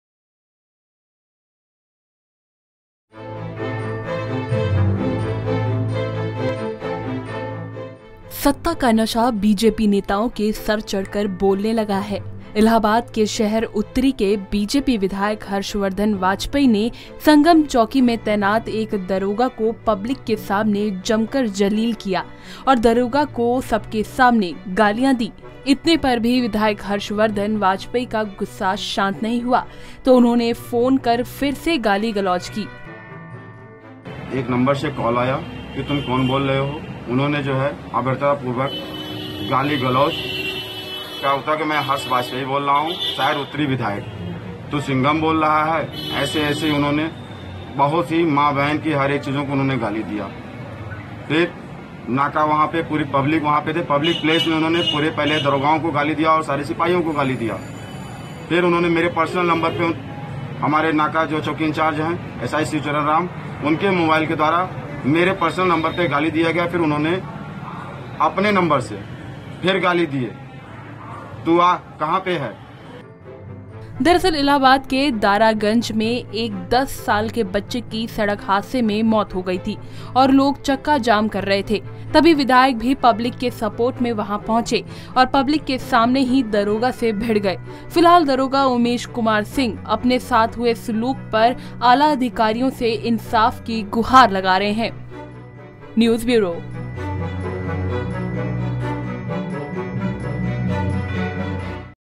बीजेपी विधायक ने दरोगा के साथ की बदतमीजी, आला अधिकारियों से इंसाफ की गुहार
सत्ता का नशा बीजेपी नेताओं के सर चढ़ कर बोलने लगा है। इलाहाबाद के शहर उत्तरी के बीजेपी विधायक हर्षवर्धन वाजपेयी ने संगम चौकी में तैनात एक दरोगा को पब्लिक के सामने जमकर ज़लील किया और दरोगा को सबके सामने गालियां दी।